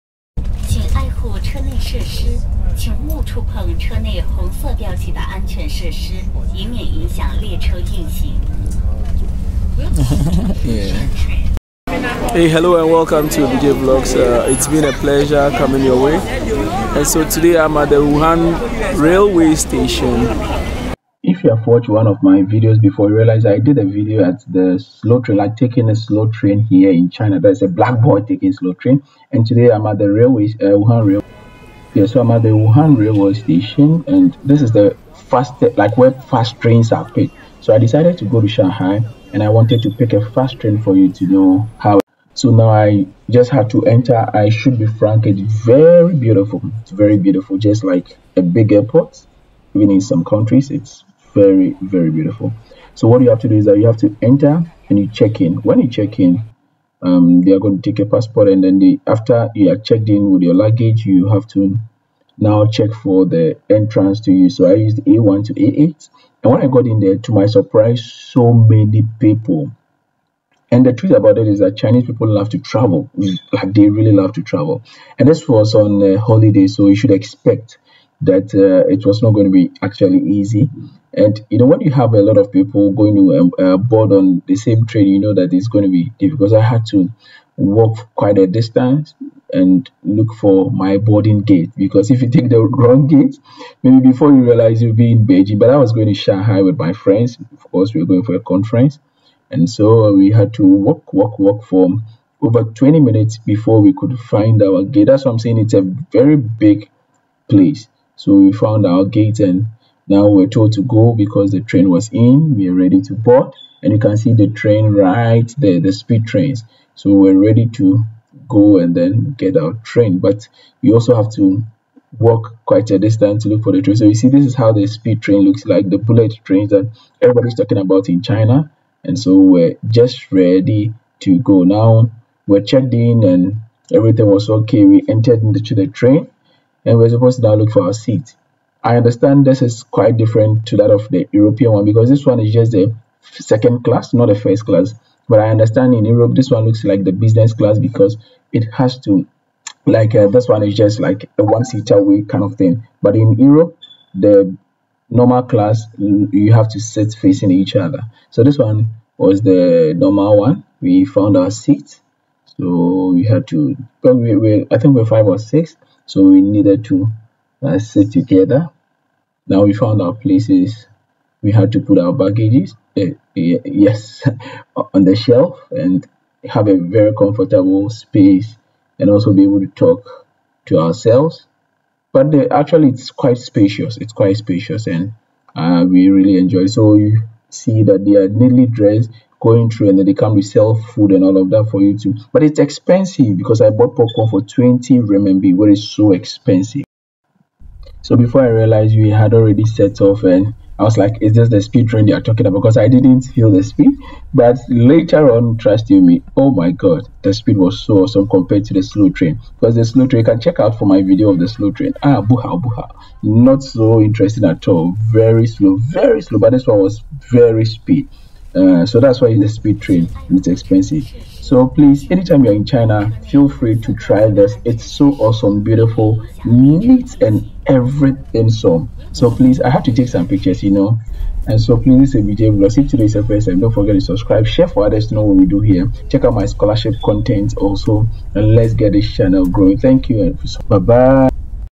The Insane High Speed Trains of China
Hello and Welcome to Abj Vlogs TV! In this exciting video, I tried the amazing High Speed train in China, traveling from the bustling city of Wuhan to the vibrant metropolis of Shanghai. The entire journey lasted for about 4 hours, and I must say, the experience was absolutely super and unforgettable. This high speed train is not just a means of transportation, but it is also a crucial and essential option for anyone planning to travel within China. It offers comfort, speed, and efficiency, making it a top choice for both locals and tourists alike.

Additionally, don’t forget to check me out on Facebook where I share more updates and content I hope you enjoy the video and find it helpful for your travel plans!